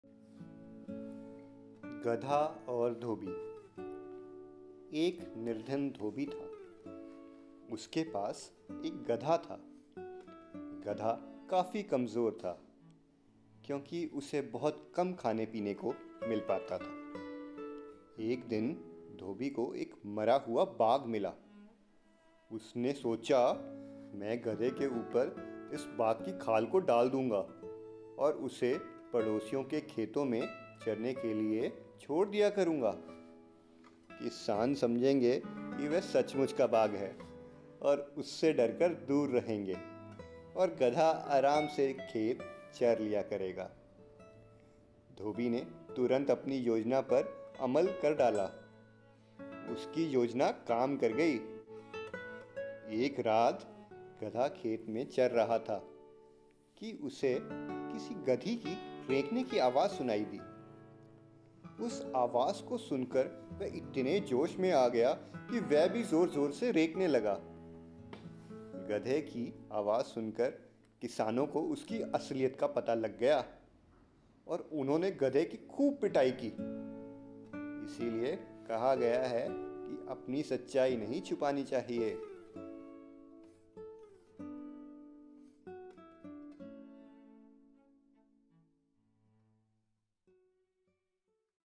Gadha Aur Dhobhi
Introducing Shubam Gupta and his first story recorded for children !